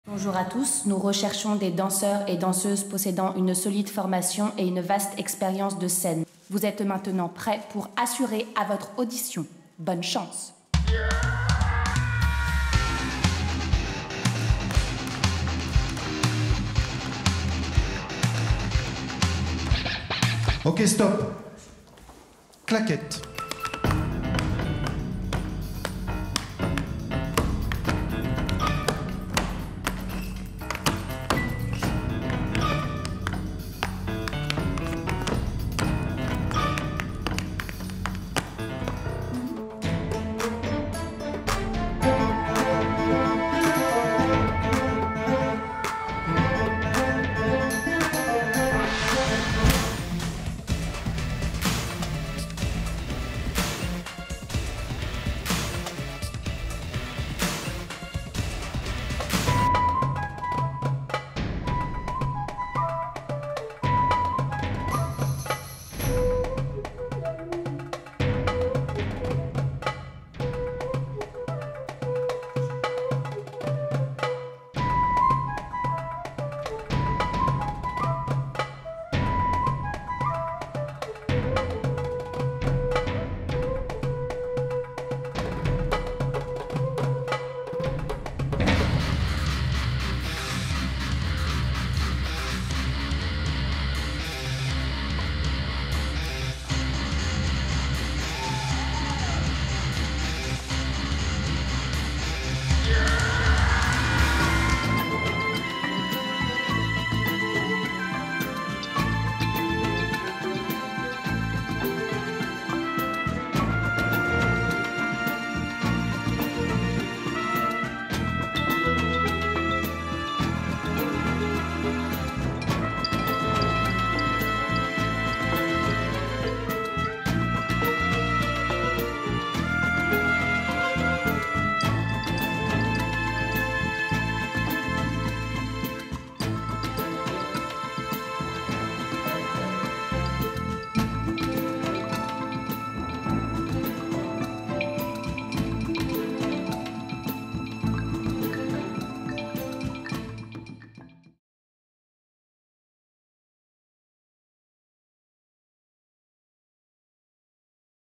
Teaser // La Finale - Josette Baïz
Création du festival Suresnes cités danse 2019

"La Finale" a inauguré la 27ème édition du Festival, avec une chorégraphie métissée et pleine d'énergie qui va du popping aux claquettes, en passant par le break, le contemporain, la new style, le smurf ou encore le krump.